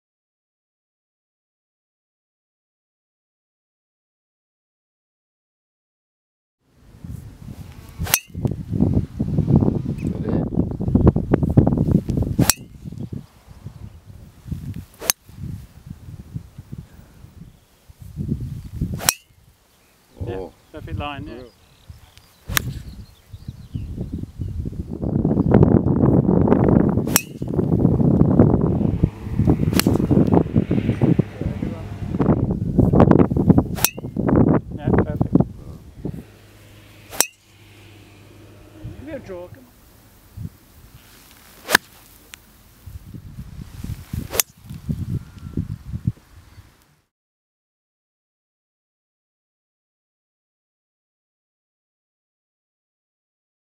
Saunton Golf Club West Course
plays the West Course at Saunton Golf Club on the Atlantic Links Tour. Playing 6 Atlantic Links courses in 5 days, while testing Adams Golf Super S Speedline Driver and Super S Irons